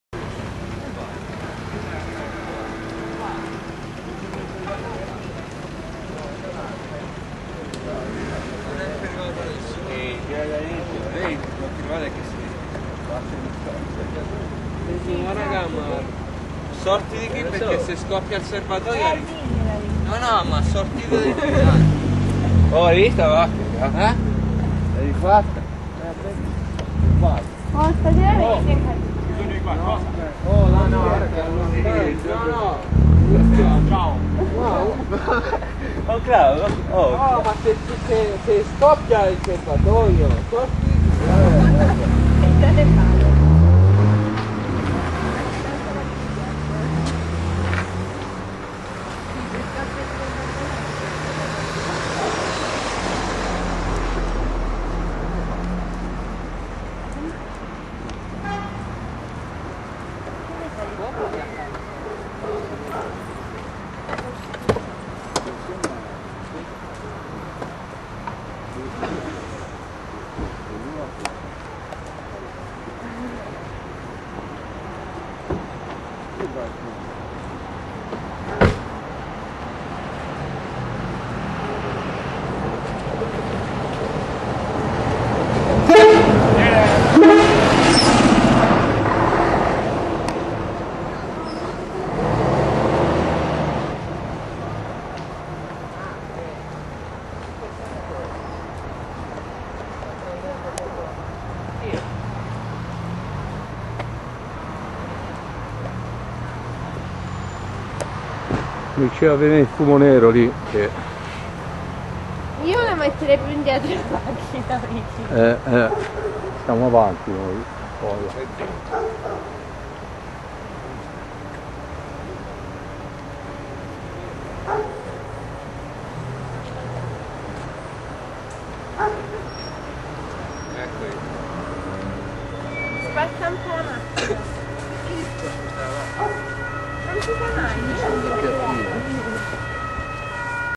AUTO PRENDE FUOCO a Pietrasanta ore 19 circa del 22 11 2012 viale Apua direzione mare
uscivo da suonare e sono rimasto bloccato da questo incendio.
AUTO PRENDE FUOCO a Pietrasanta ore 19 circa del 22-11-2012 viale Apua direzione mare. all'altezza zona Macelli